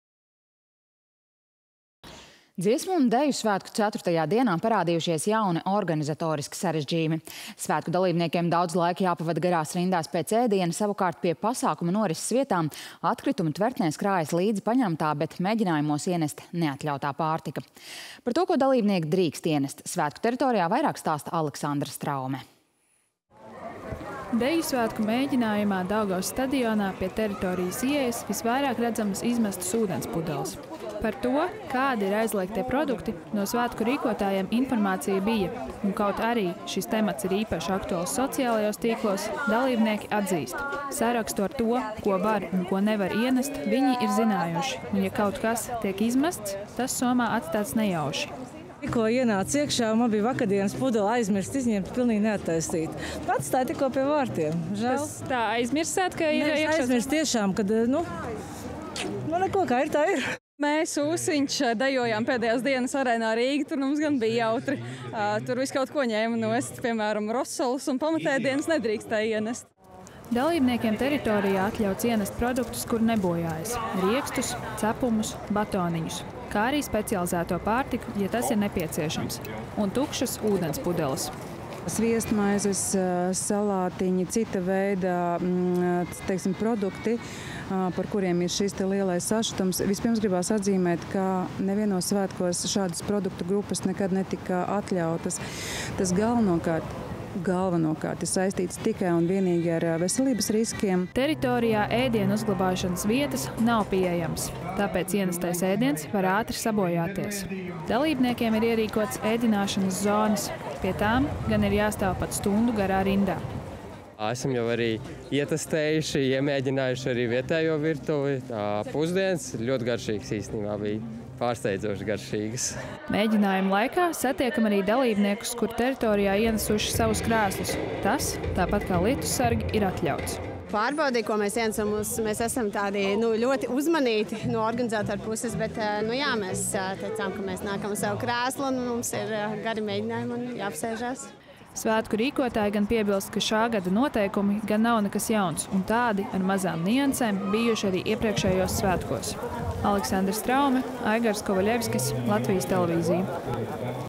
Ko Dziesmu svētku dalībniekiem liegts ienest teritorijā
Garas rindas pēc ēdiena un aizliegtie ienesamie produkti. Dziesmu un deju svētku ceturtajā dienā parādījušās organizatoriskas problēmas. Par to, ko dalībnieki drīkst ienest svētku teritorijā, vairāk stāsta Aleksandra Straume.